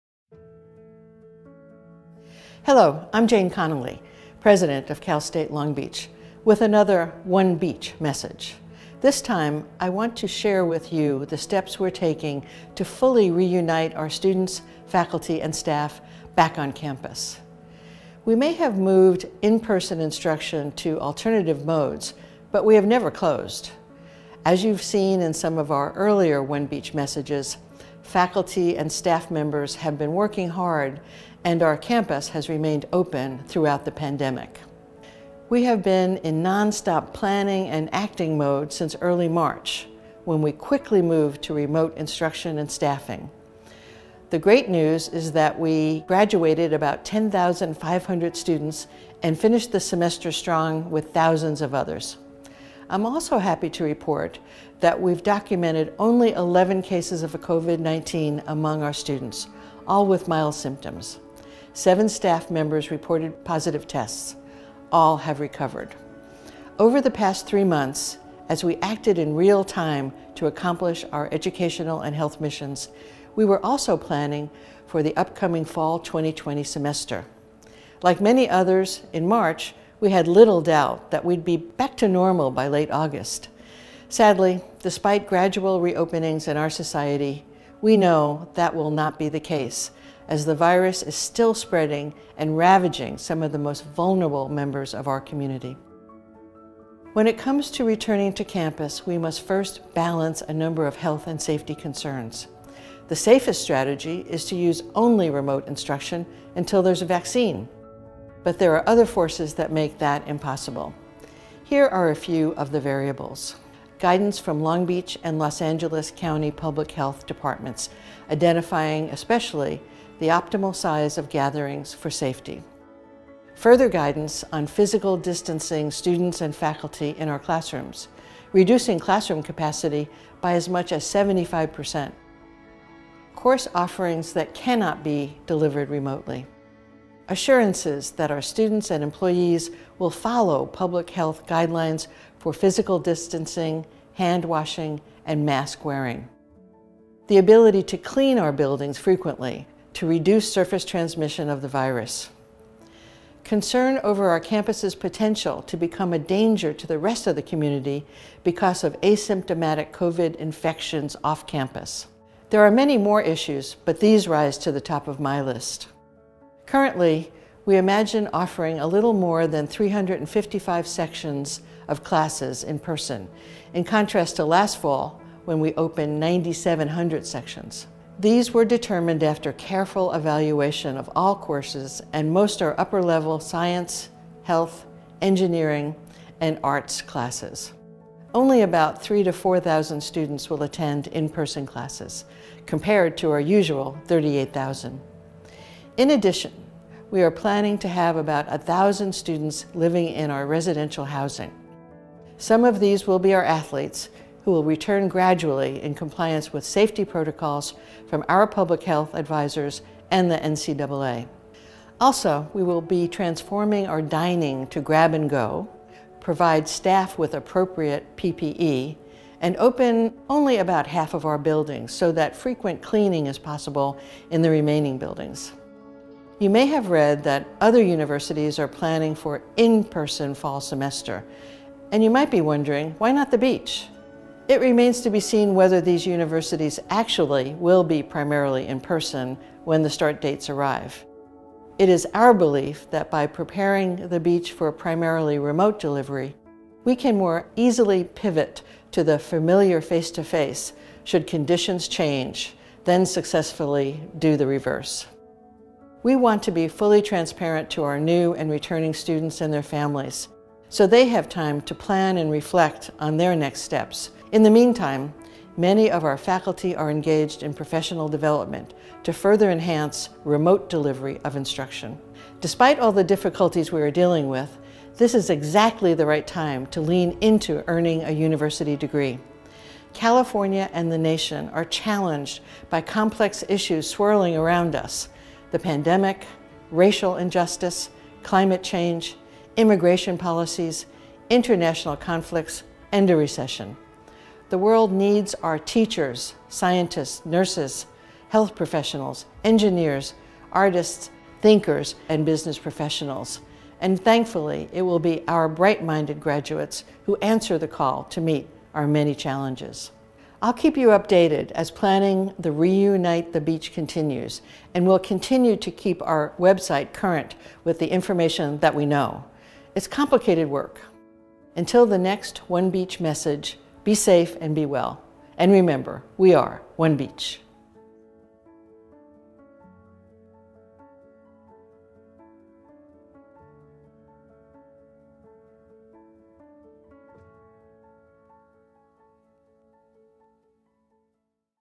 OneBeach - Reuniting The Beach
In this OneBeach video, CSULB President Jane Close Conoley shares the steps being taken to fully reunite our students, faculty, and staff back on campus.

When it comes to returning to campus, we must first balance a number of health and safety concerns.